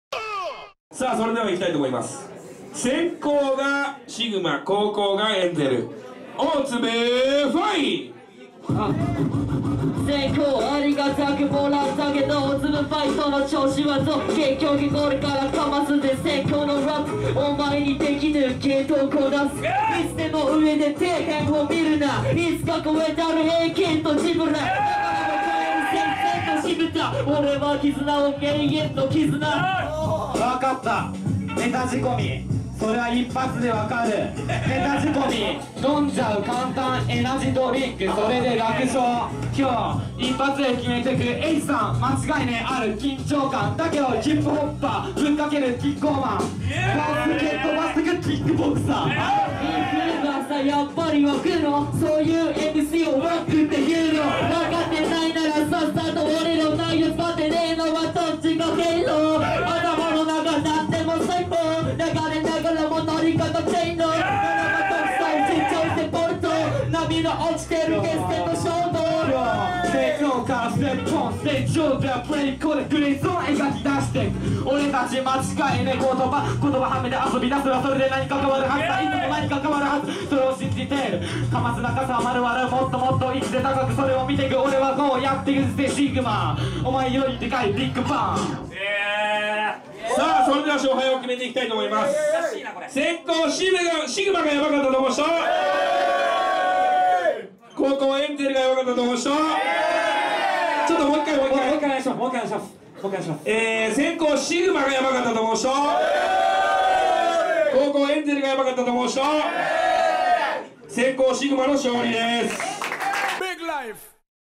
大粒FIGHT vol.17 Σ(シグマ) vs ENZEL
大粒FIGHTvol.17 全動画アップしました🙌

この回も頭から通しで見て欲しい素晴らしい内容になってます！

見終わった後ケツに火がついた方は、2017年8月6日(日)16時半に池袋knotで当日エントリーの抽選を行うので、宜しくお願いします！

大粒FIGHTアカウント(Twitter)